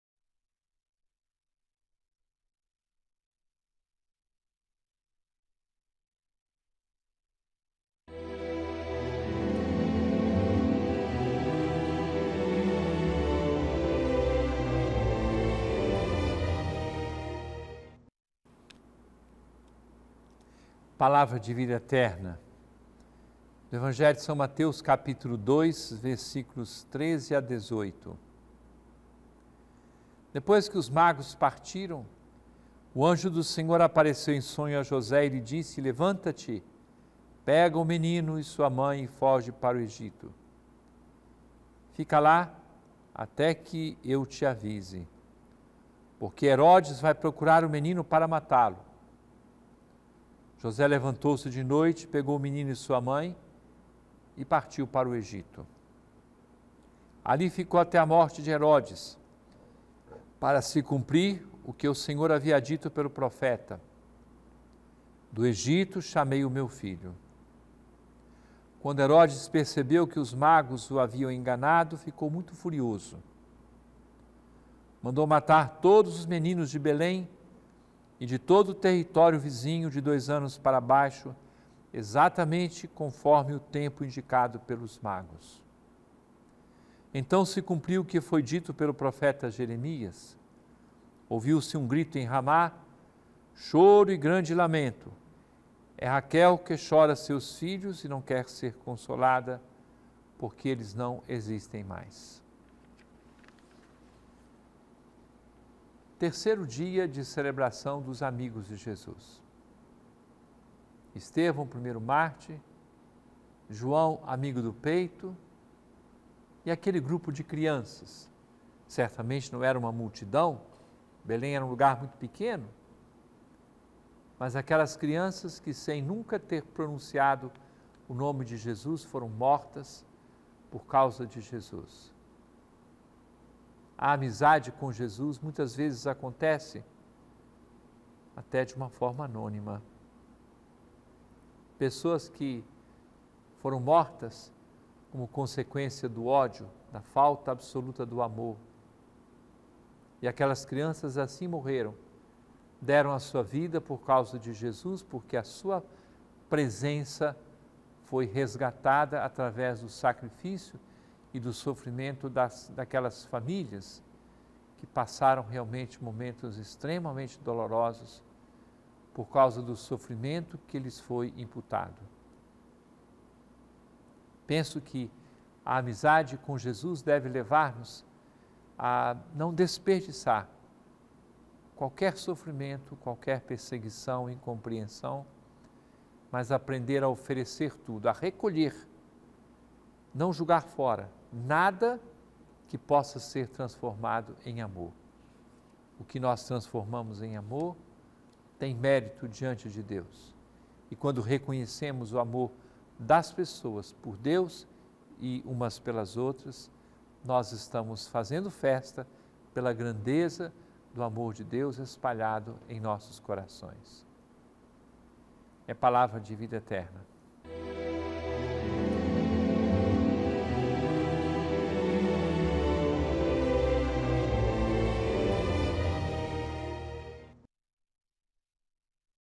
Palavra de Vida Eterna | Mateus 2, 13 - 18 - 28/12/2019
13Depois que os magos partiram, o Anjo do Senhor apareceu em sonho a José e lhe disse: "Levanta-te, pega o menino e sua mãe
e foge para o Egito! Fica lá até que eu te avise! Porque Herodes vai procurar o menino para matá-lo". 14José levantou-se de noite,
pegou o menino e sua mãe, e partiu para o Egito. 15Ali ficou até à morte de Herodes, para se cumprir o que o Senhor havia dito pelo profeta: "Do Egito chamei o meu Filho". 16Quando Herodes percebeu
que os magos o haviam enganado, ficou muito furioso. Mandou matar todos os meninos de Belém e de todo o território vizinho,
de dois anos para baixo, exatamente conforme o tempo indicado pelos magos. 17Então se cumpriu o que foi dito pelo profeta Jeremias: 18"Ouviu-se um grito em Ramá, choro e grande lamento:
é Raquel que chora seus filhos, e não quer ser consolada, porque eles não existem mais".